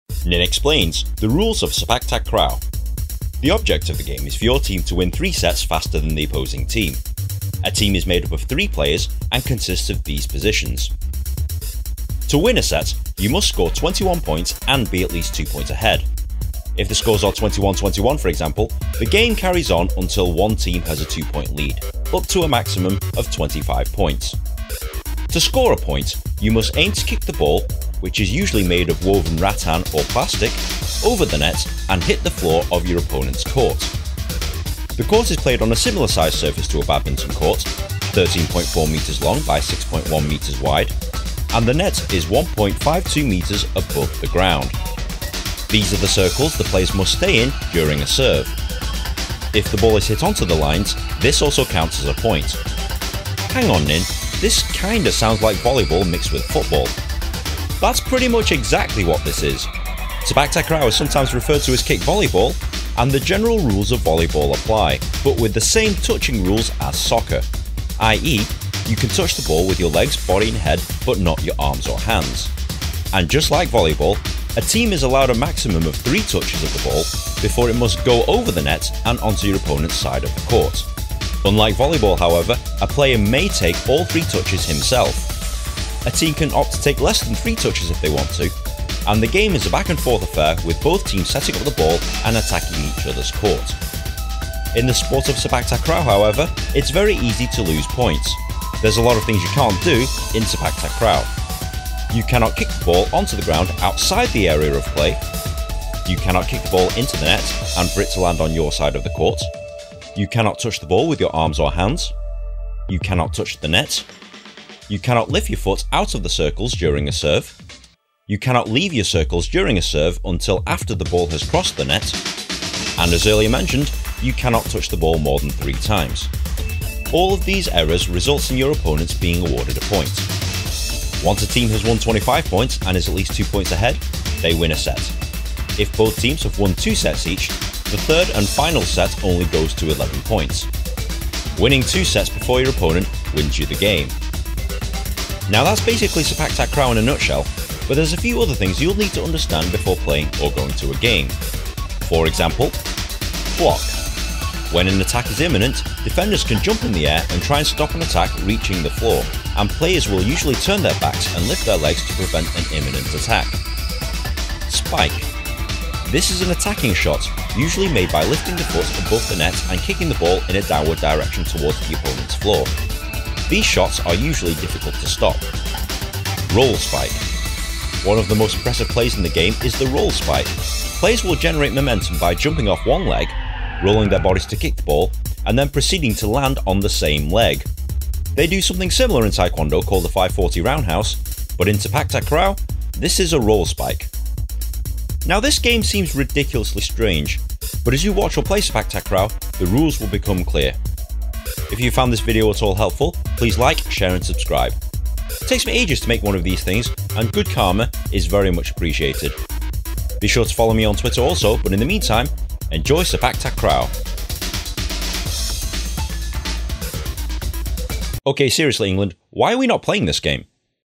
The Rules of Sepak Takraw - EXPLAINED!
Ninh explains the Rules of Sepak Takraw, a strangely popular sport played worldwide particularly in East Asia.
A beginner's explanation of Sepak Takraw, otherwise known as ‘Kick Volleyball’.
Watch this short video tutorial guide on how to play Sepak Takraw under International and ISTAF rules.
Learn about tekong, regus, sets, block, spike, roll spike, kick, highlights, and more!